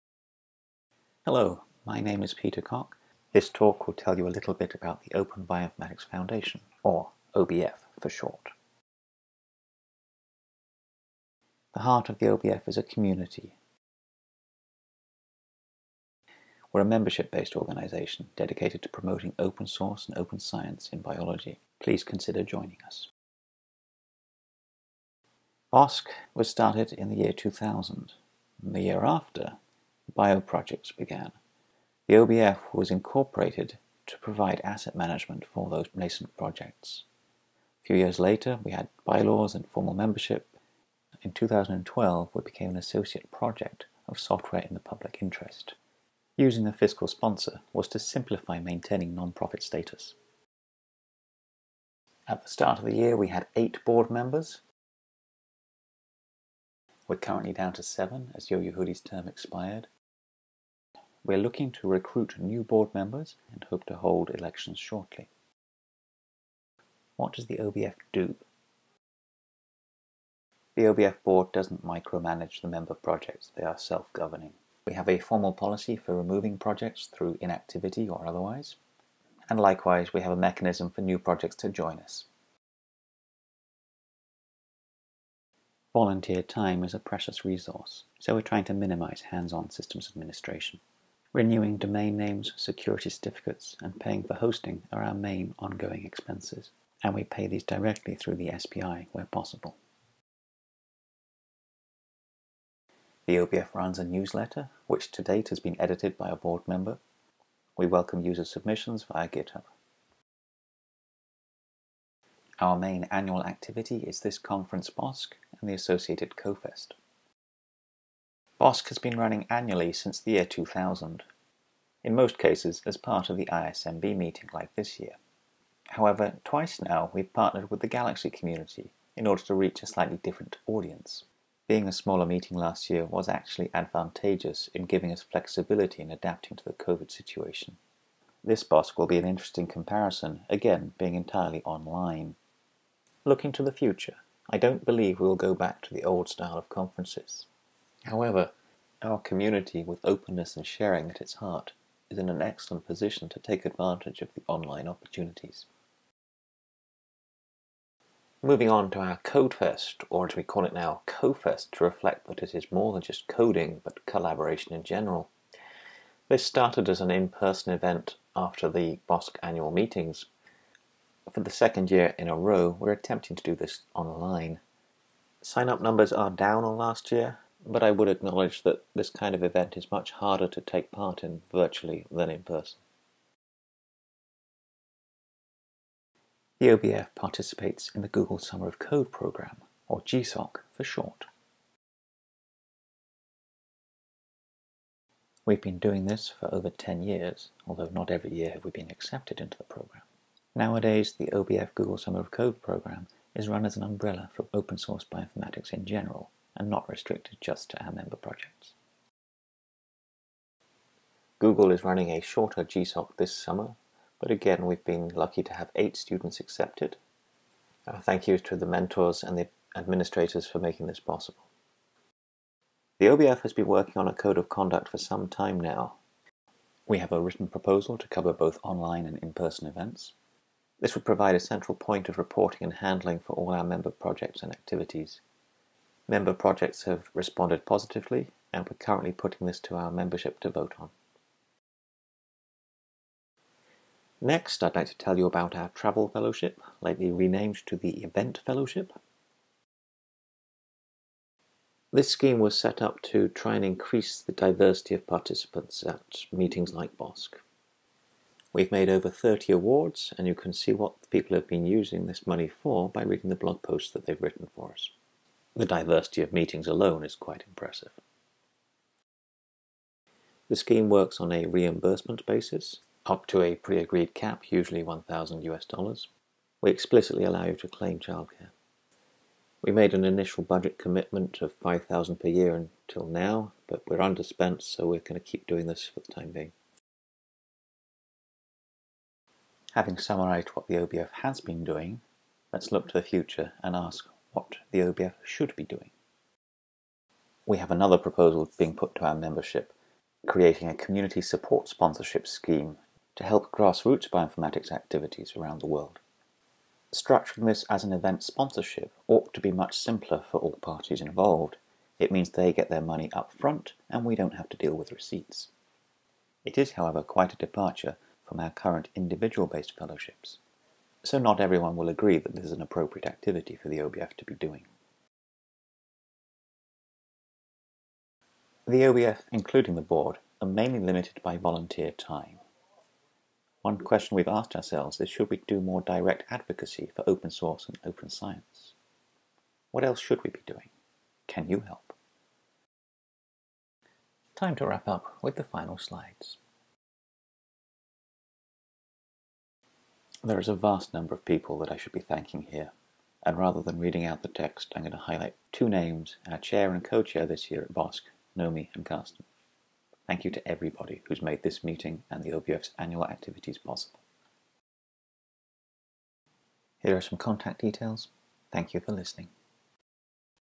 BOSC 2021 Peter Cock, OBF Update
Presented at the Bioinformatics Open Source Conference (BOSC) 2021 which was held online as part of ISMB/ECCB